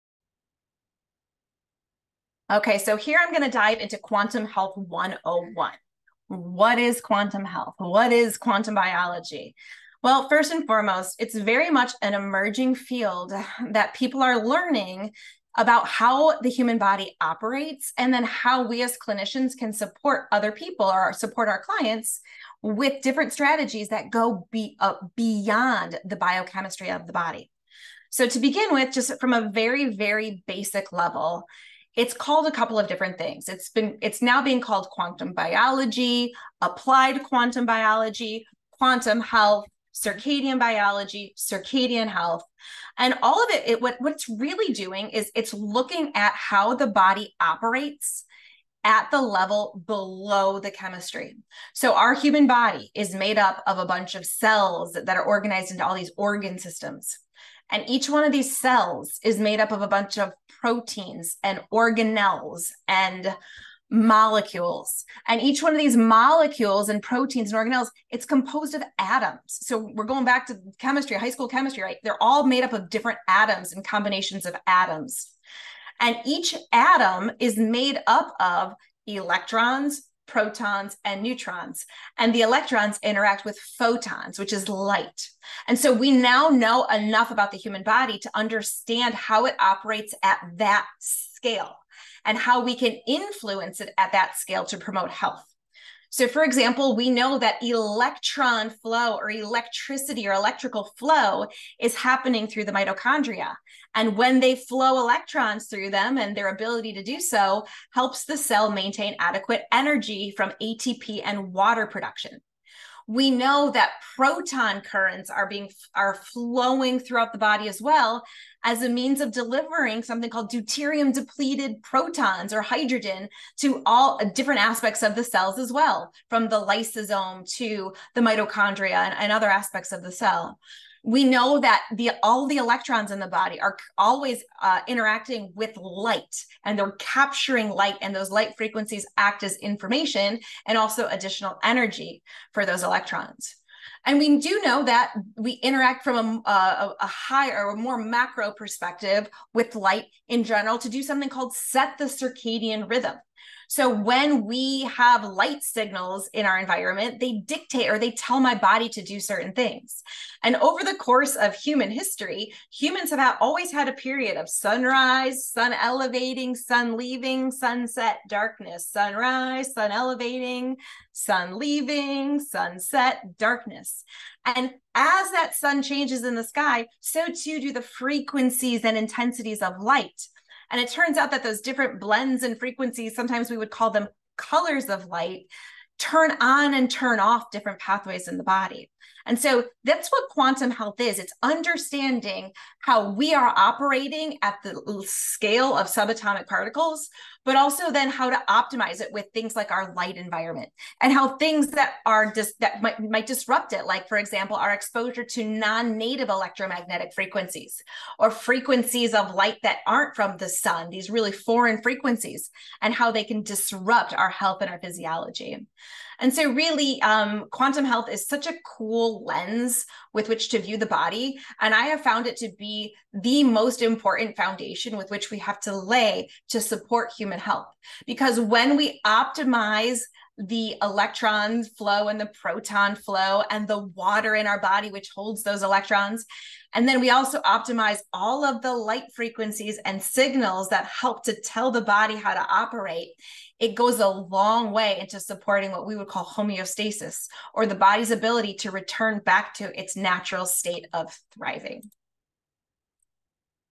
Quantum Health 101: What is Quantum Health?
If you're new to "quantum health" or have just struggled to communicate what it is when talking to your family and friends about it, I wanted to spend some time today going back to basics. Watch this short video for a primer on quantum health.